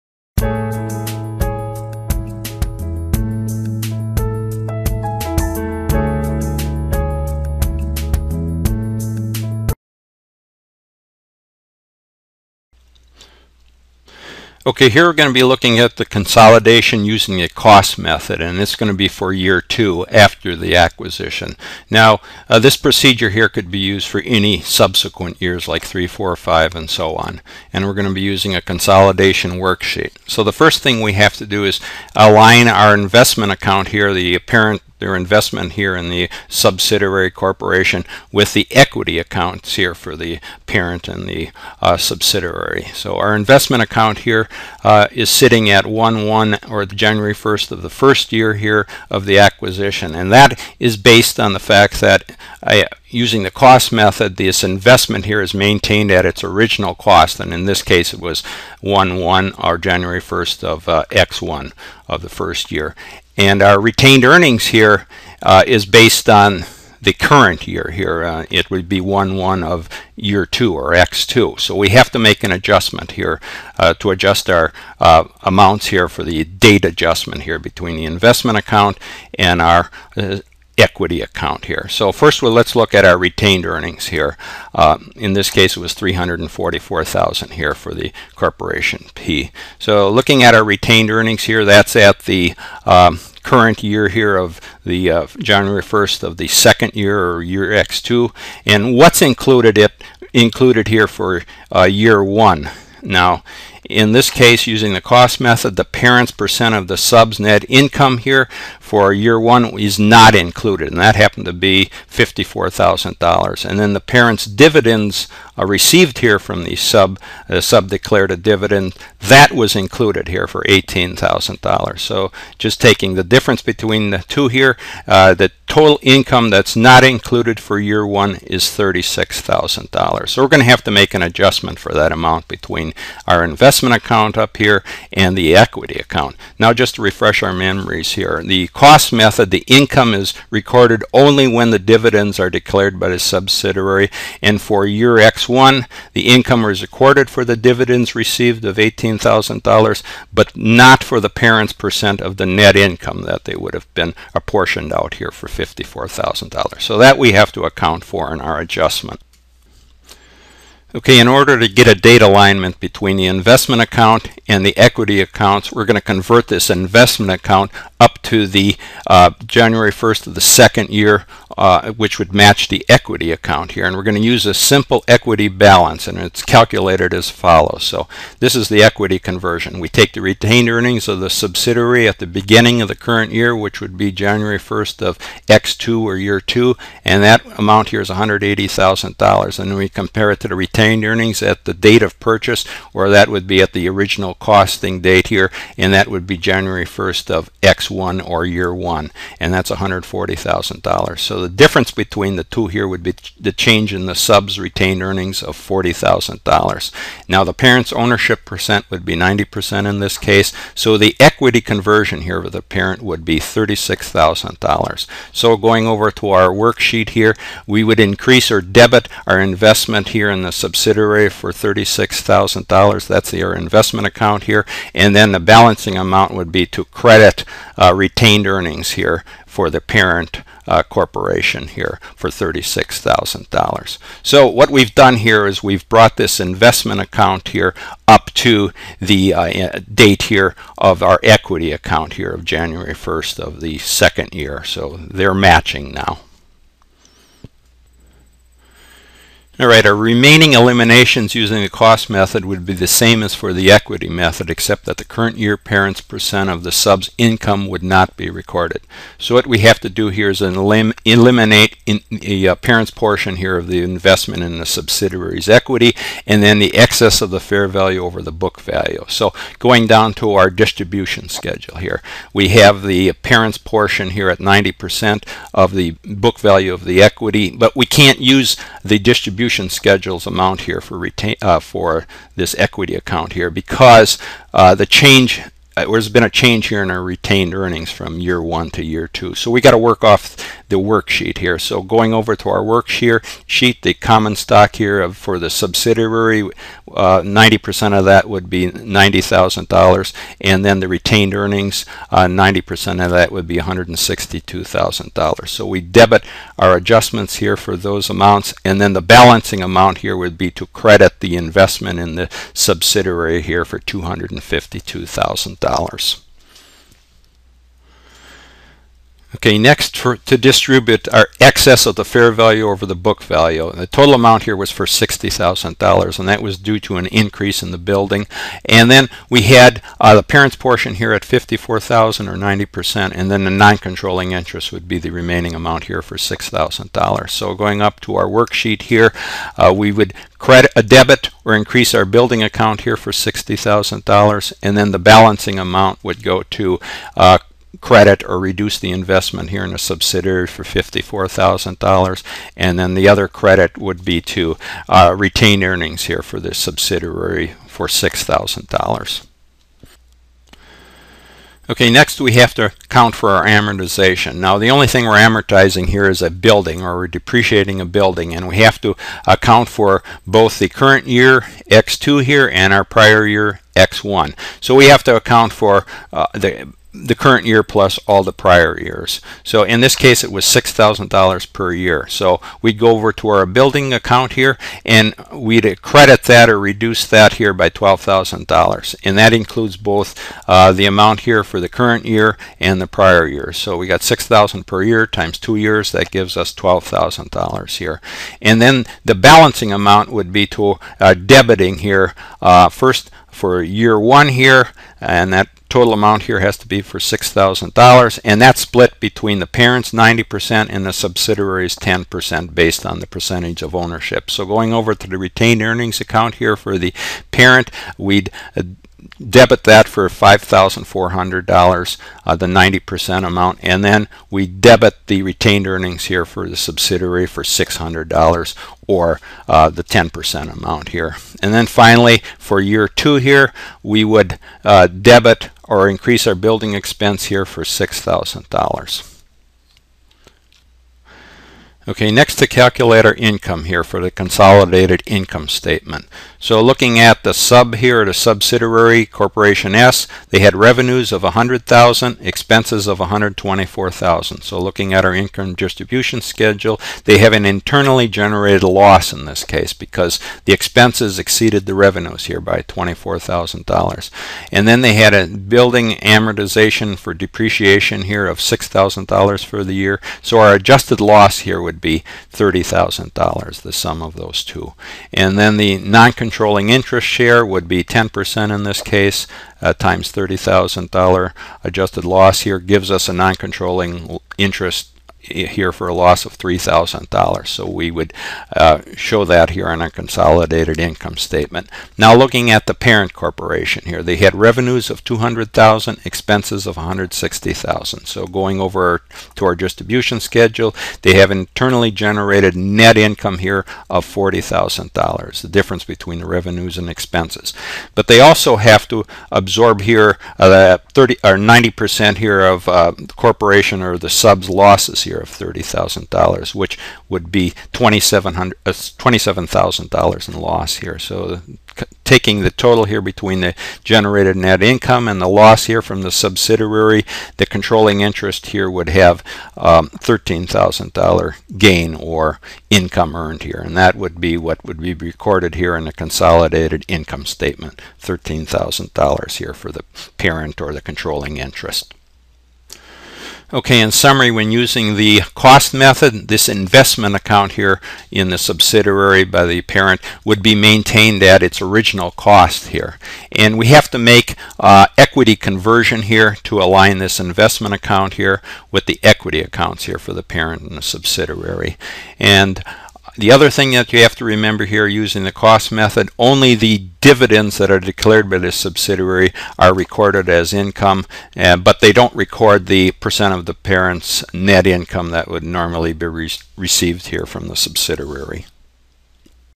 Consolidate Cost Method For Subsequent Years (Example Based On Year Two)
Consolidation procedures using the cost method, parent company will be consolidated with subsidiary company, first year consolidation after acquisition has been completed now the consolidation is being made for the second year, example will be based on the adjustments and eliminations that are completed on a consolidation worksheet, aligning the equity account with the investment account for the same point in time, includes the eliminations and adjustments required, determination and distribution schedule, etc. detailed procedure with calculations and accounting example by Allen Mursau